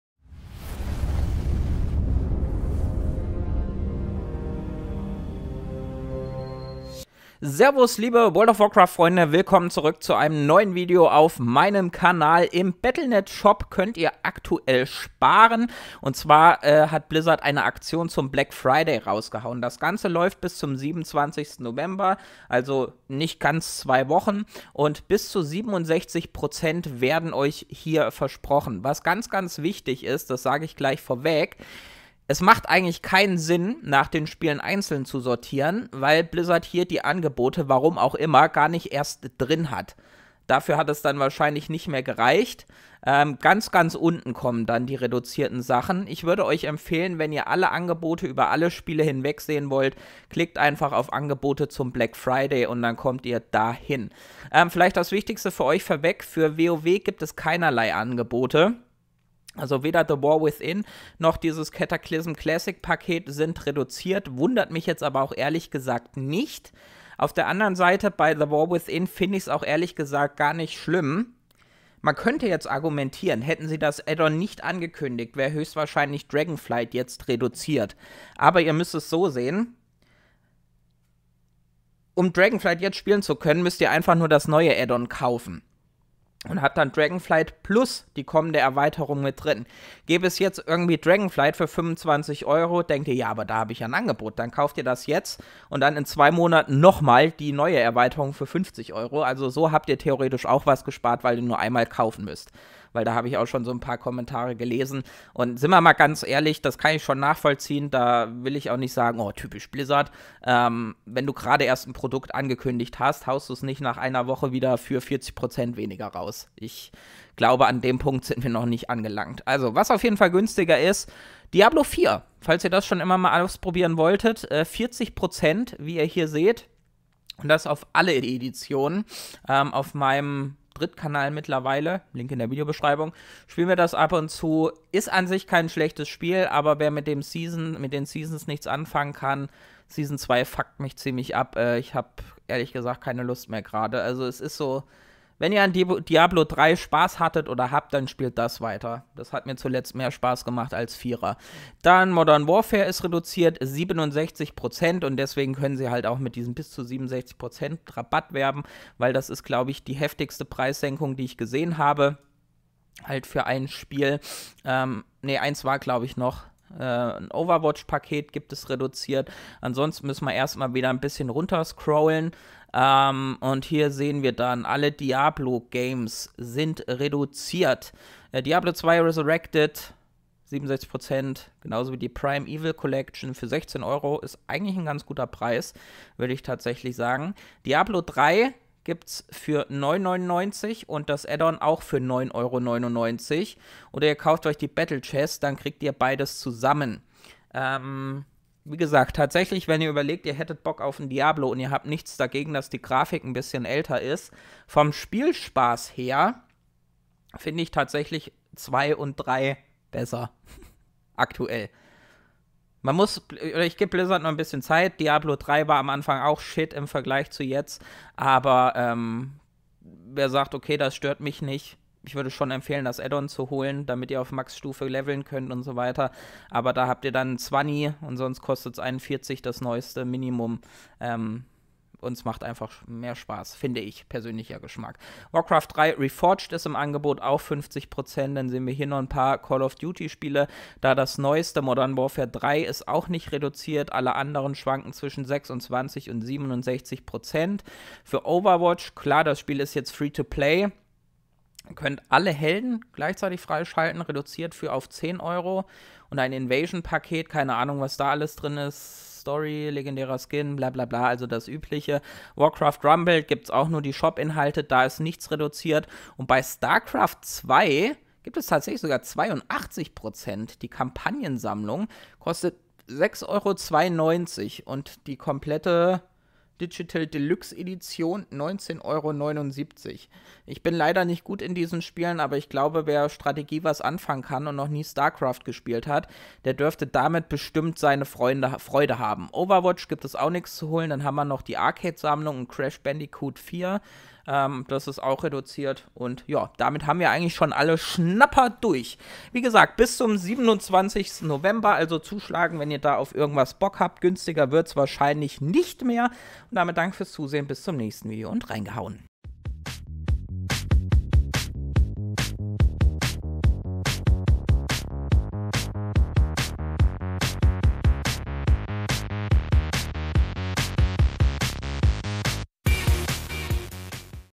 Bis zu 67 Prozent: Black Friday im Battle.net mit Angeboten für viele Spiele
Auch Blizzard haut zum Black Friday einige Angebote raus und gibt euch zahlreiche Spiele im Battle.net zu einem günstigeren Preis.

Beste Pflegeprodukte für Männer - von Dick Johnson!
20% sparen mit dem Code Geezax20!